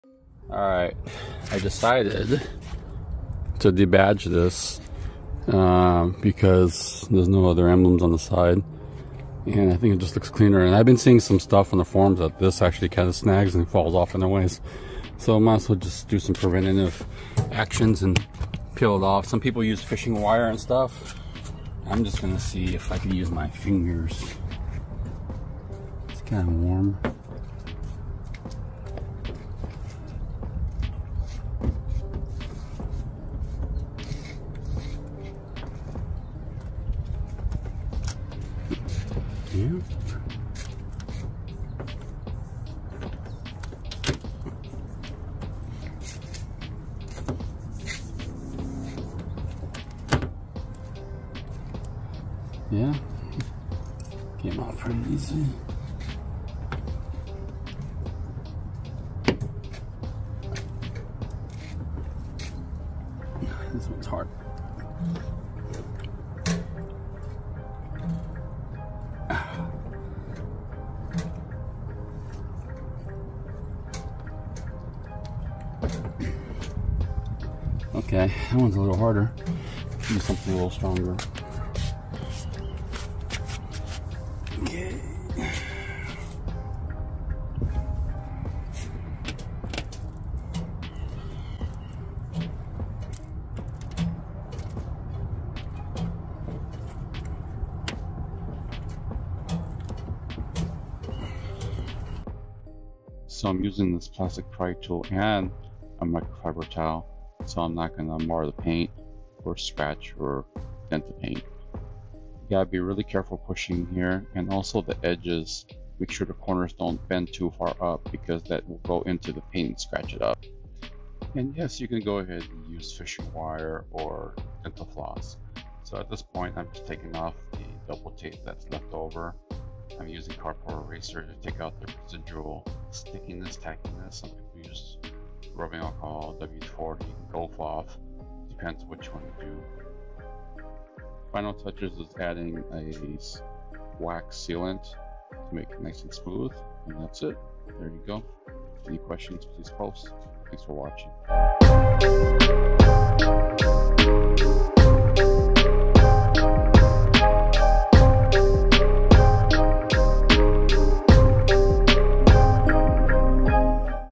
Removing Emblems Debadge the HARD Way 😂
So I decided to remove the Mach E badges the hard way lol.

I usually have better luck than this

some things used in this video
Carpro Eraser
microfiber towels
pry tool
great polish and sealant

If you find these videos helpful and would like to support me and my work, see below: